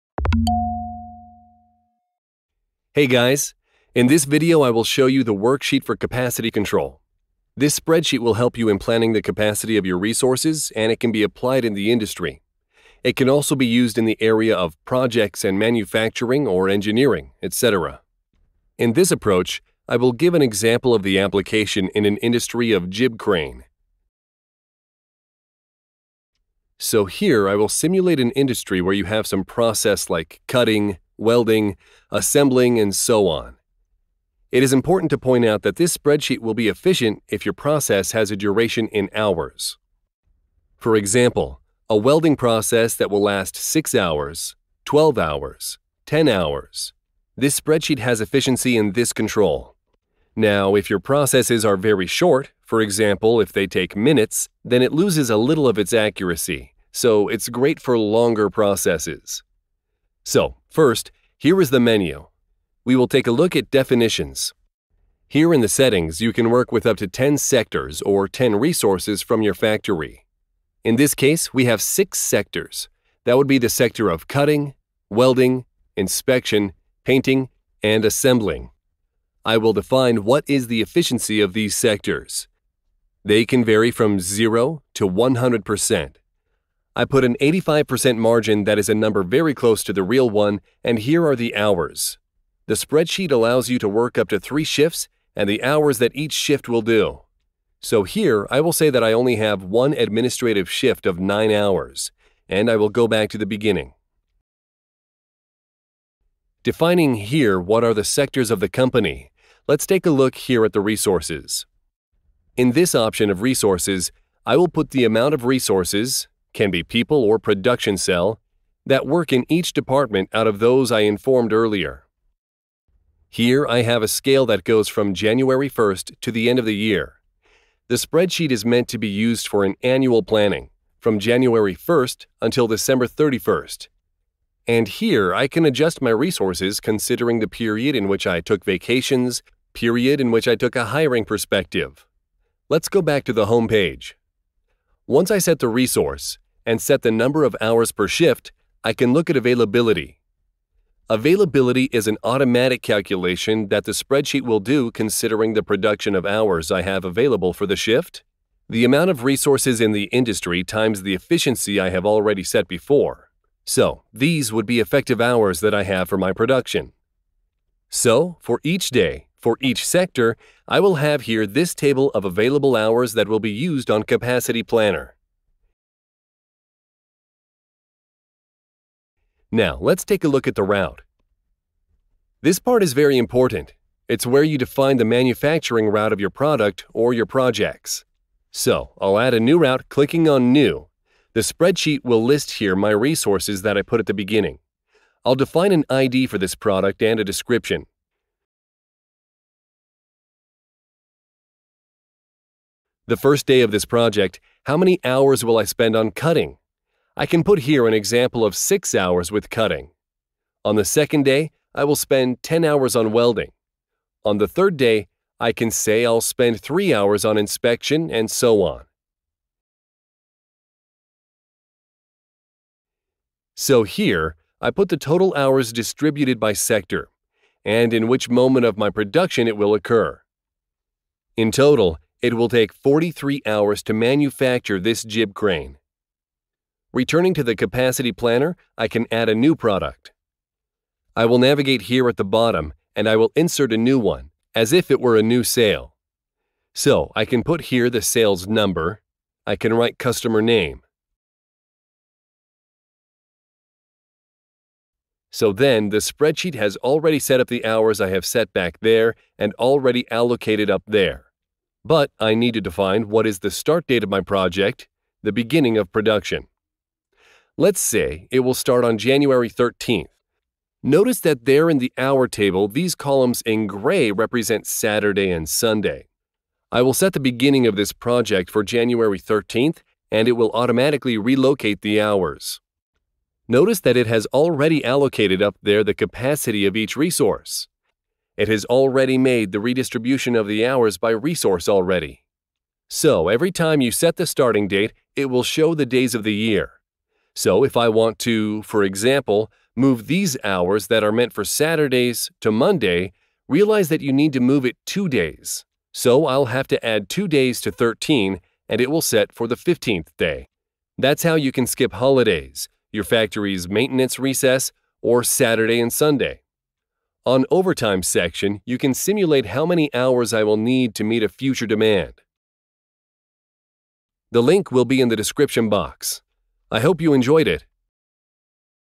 Capacity Planning Excel Template
In this video I will show you the worksheet for capacity control. This spreadsheet will help you in planning the capacity of your resources and it can be applied in the industry. It can also be used in the area of projects and manufacturing or engineering, etc.